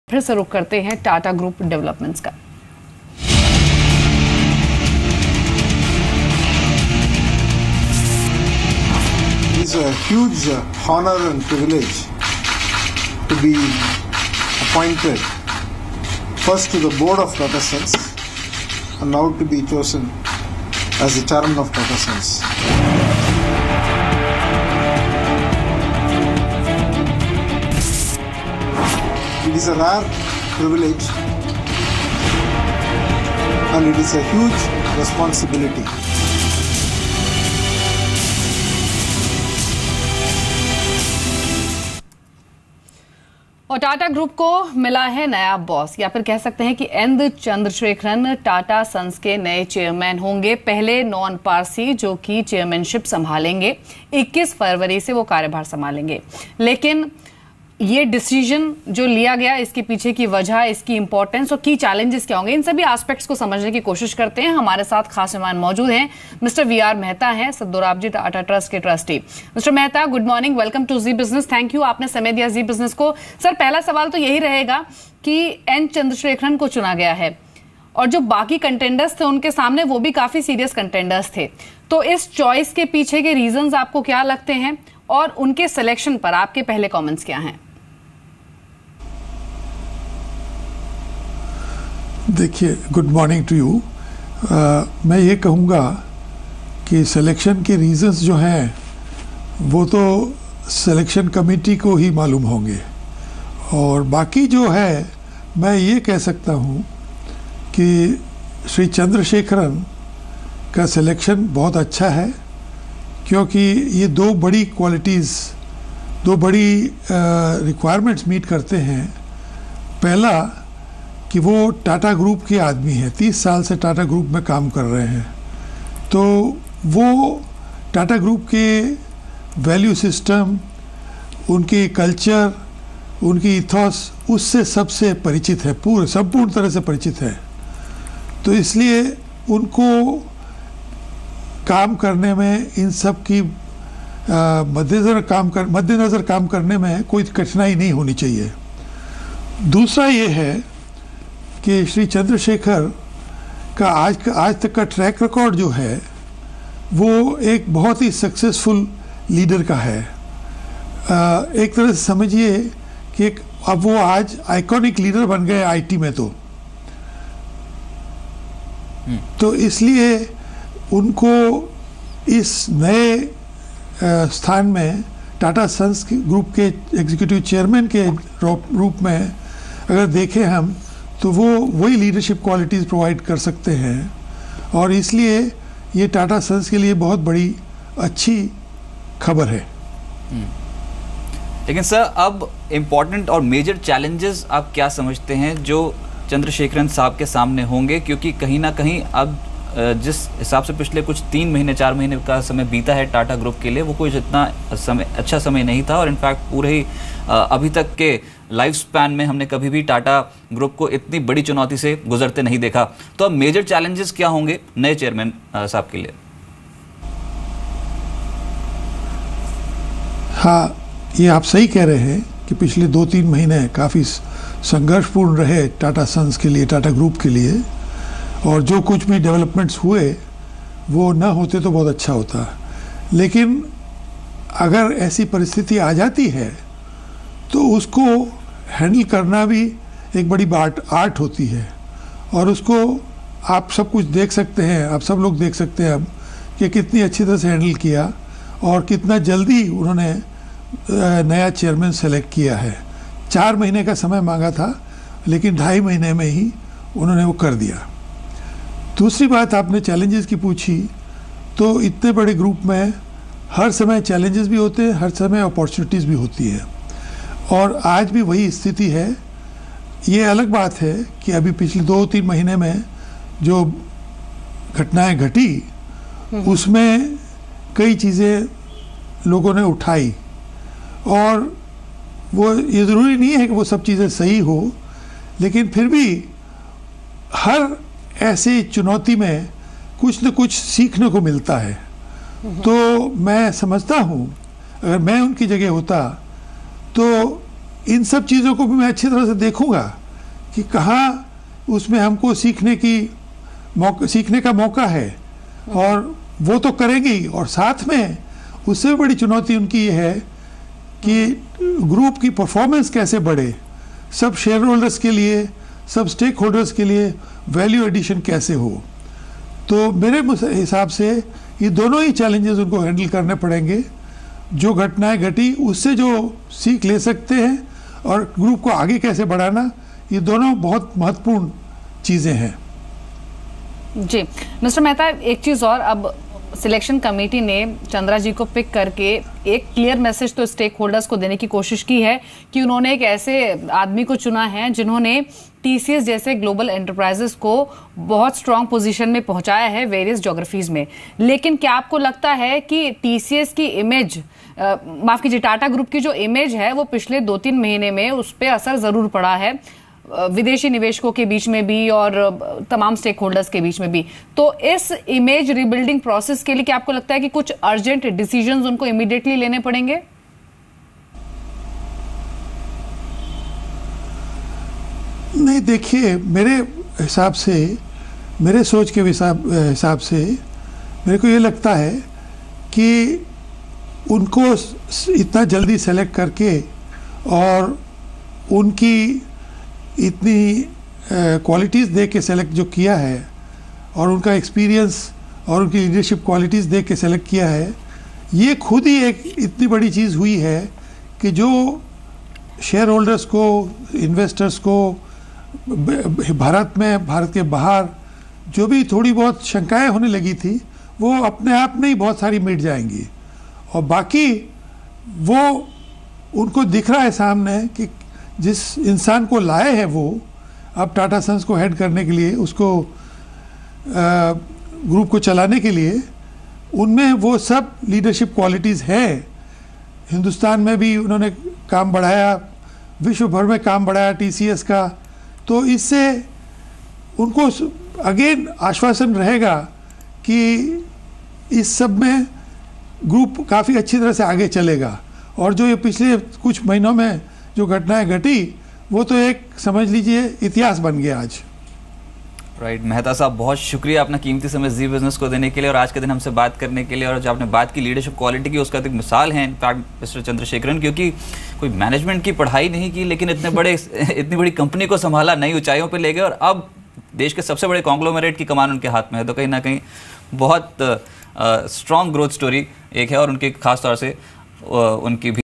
Conversation with V.R. Mehta, trustee, Dorabji Tata Trust on N. Chandrasekaran's appointment
Exclusive conversation with V.R. Mehta, trustee, Dorabji Tata Trust on appointment of N. Chandrasekaran. Watch the complete news story of First Trade to get detailed updates on the stock market, shares and their recent trends, updates on the market!